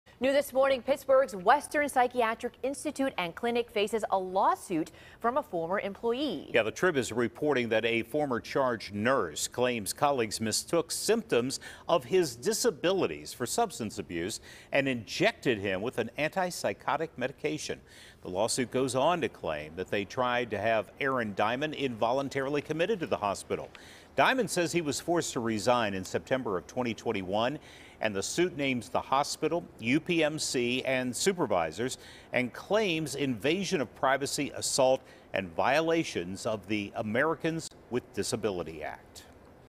Former nurse files suit against Western Psych
The former charge nurse claims that the supervisors tried to have him involuntarily committed.